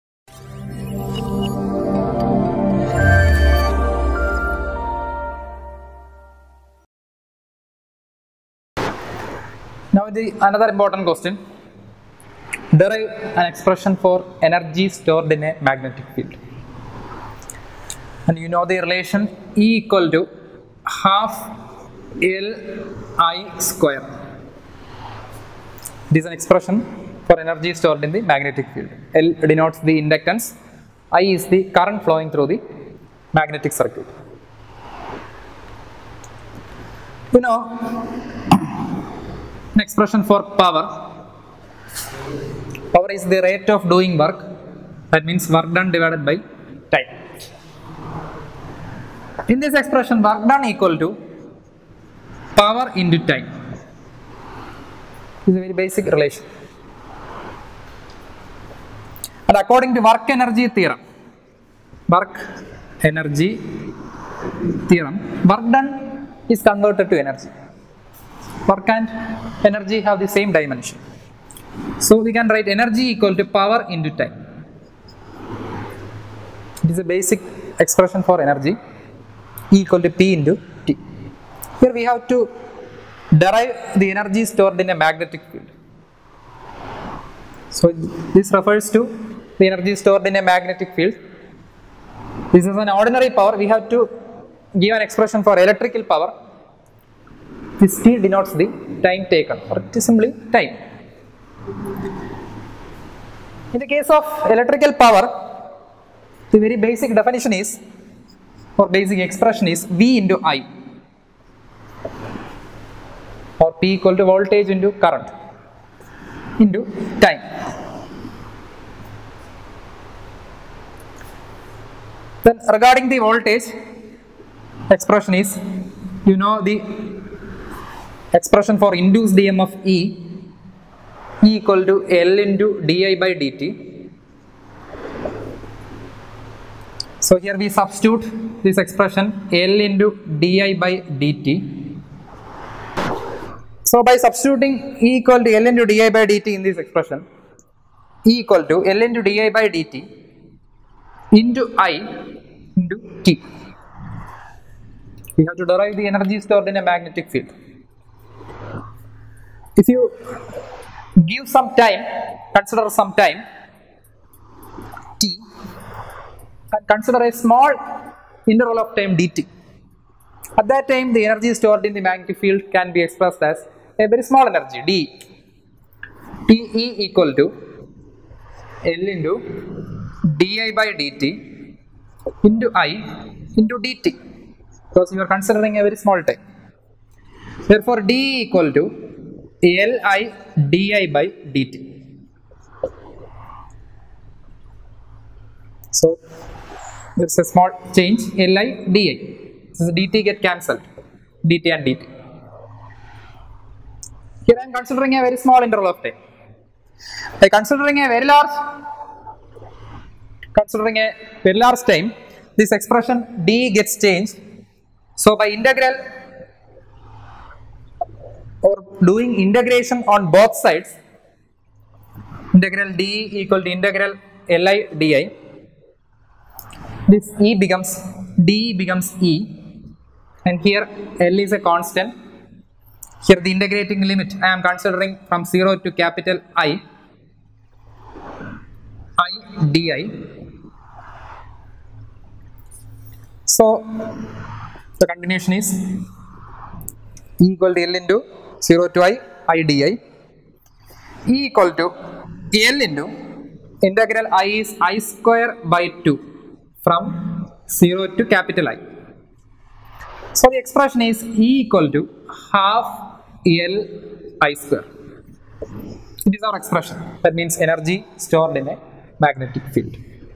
Energy stored in a magnetic field | Engineering Lectures | Electrical | M2 | Btech | KTUGuru | KTU
Resource person: Rahul  Charles
Assistant Professor Department of EEE, College of Engineering Muttathara

Cameraperson: Harikrishnan
Our WhatsApp communities:-

This video is by KTUGuru.